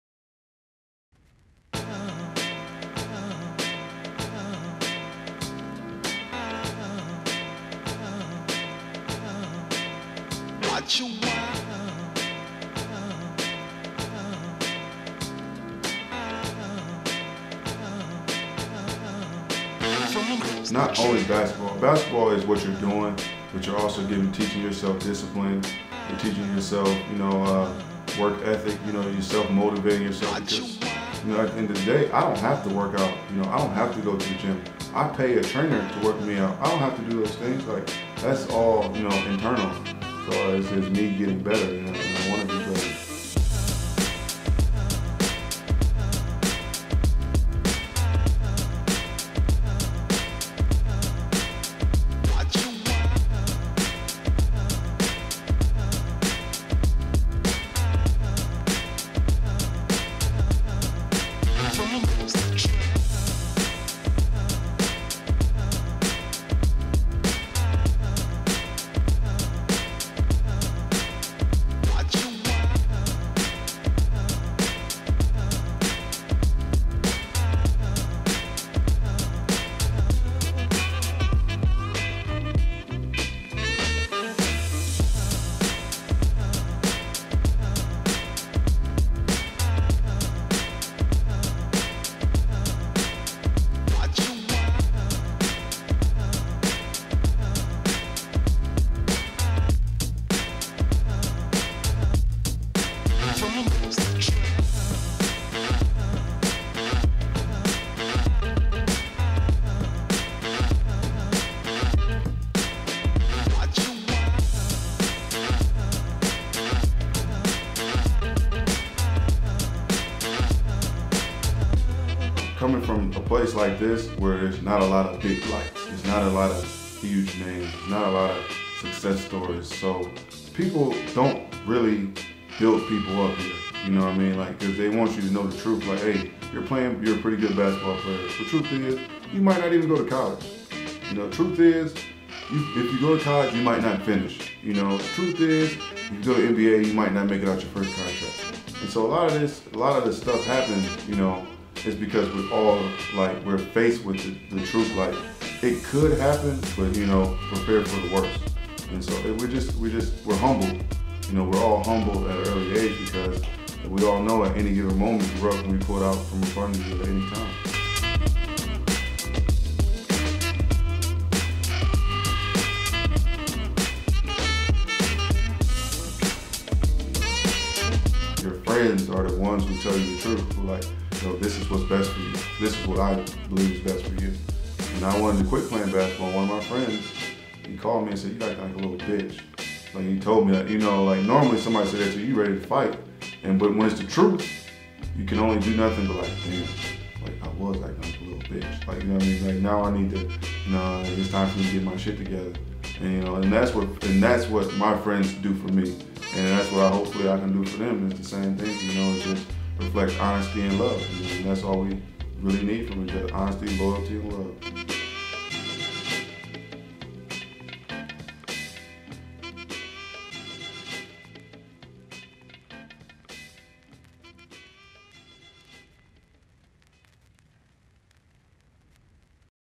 The Off-Season Workout That Brought Lebron Back To Cleveland [Romeo Travis | Episode 1: Mixtape]
While everyone spent last summer talking about hairlines, the "Kings of Akron" focused on greatness.

Credits:
Edited by Tom Yang